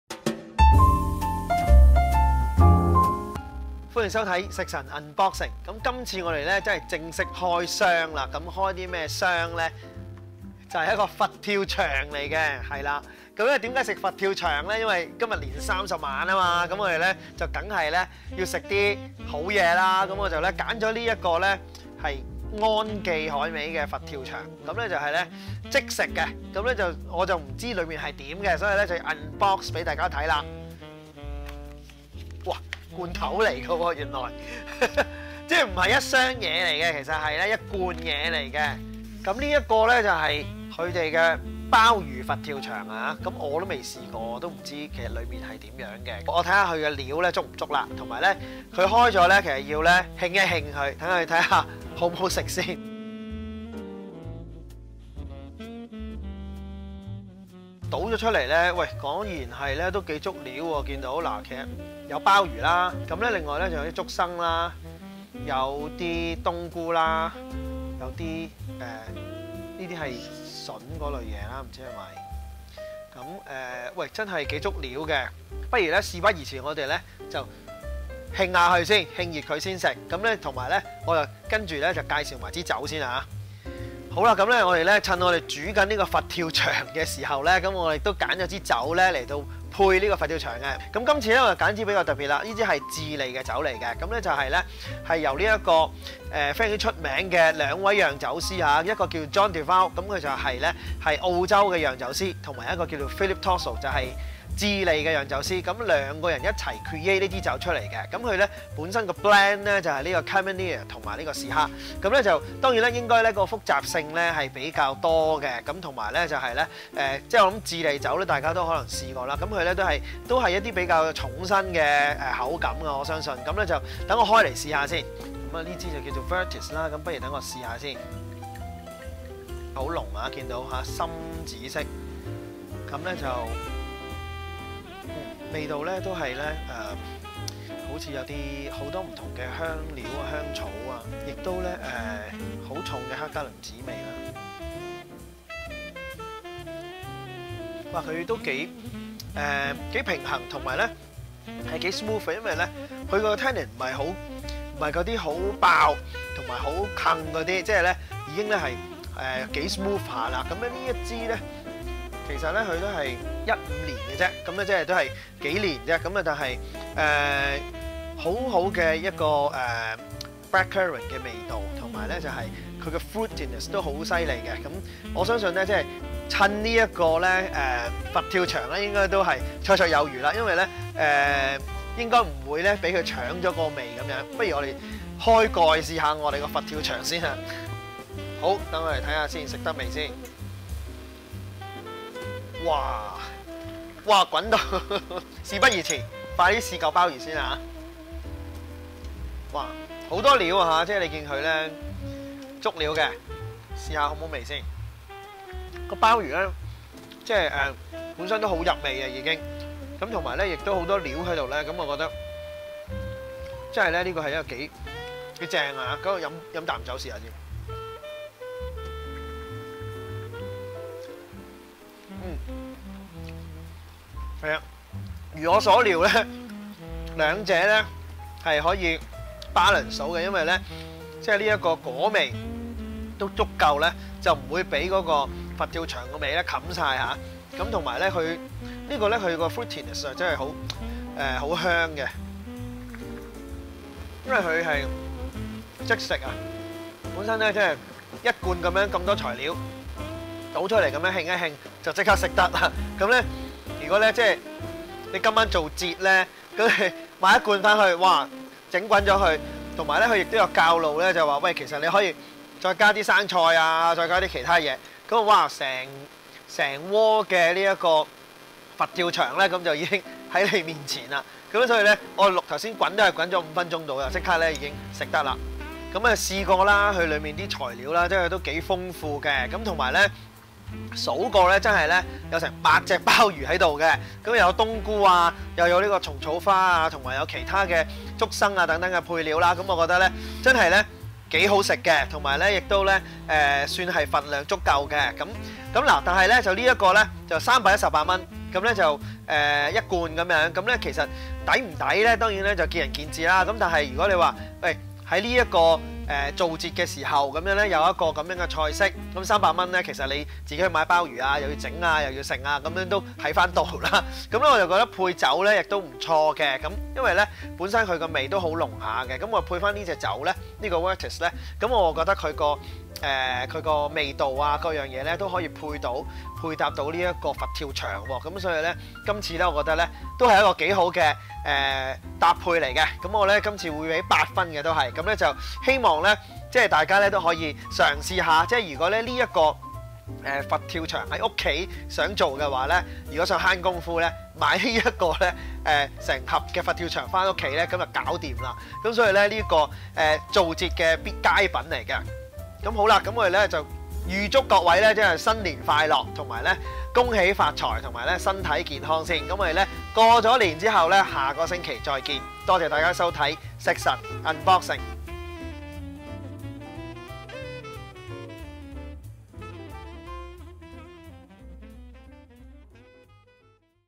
「佛跳牆」原來都有得開箱?! 團年飯又啱唔啱食？【食臣.開箱 |EATSON.UNBOXING】
第三集：安記頂級鮑魚佛跳牆
Episode 3: ON KEE DELUXE ASSORTED SEAFOOD IN CHICKEN SOUP

佛跳牆原來都有得開箱?! 今次「食臣」試即食佛跳牆，仲用智利葡萄酒VERTICE搭配！到底即食佛跳牆質素如何？裏面又足唔足料呢？團年飯又啱唔啱食？馬上去片⋯⋯